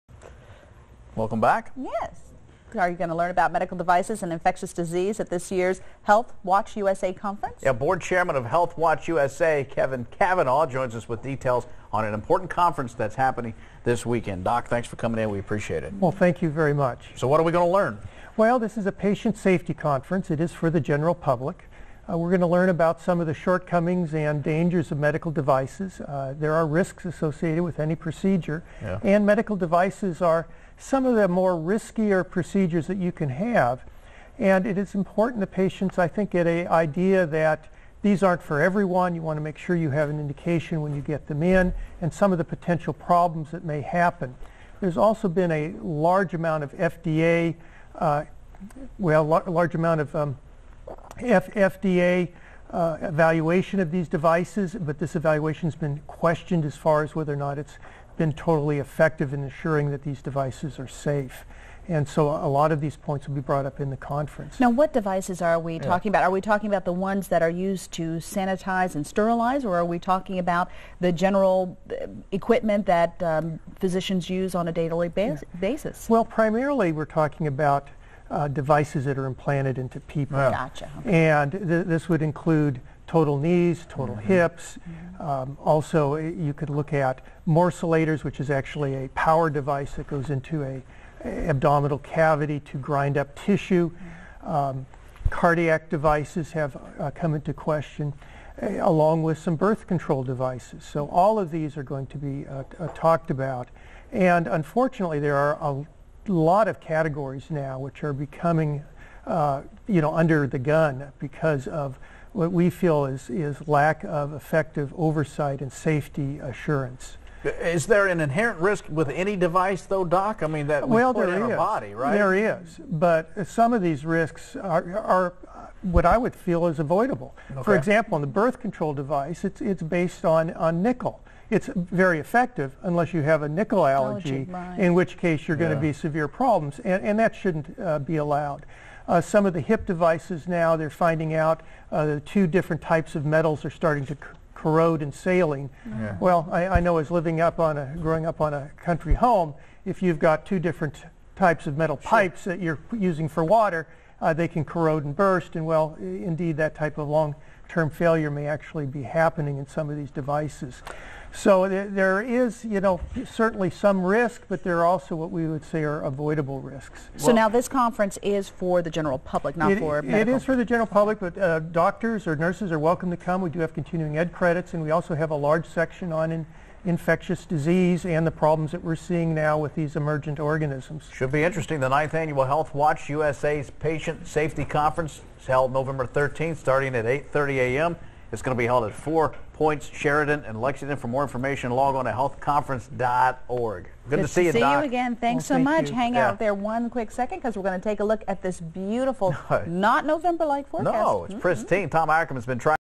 Health Watch USA
Dr. Kevin Kavanagh talks with Lee and Dia on LEX 18 News at 12:30 about the conference coming up at the Four Points Sheraton on November 13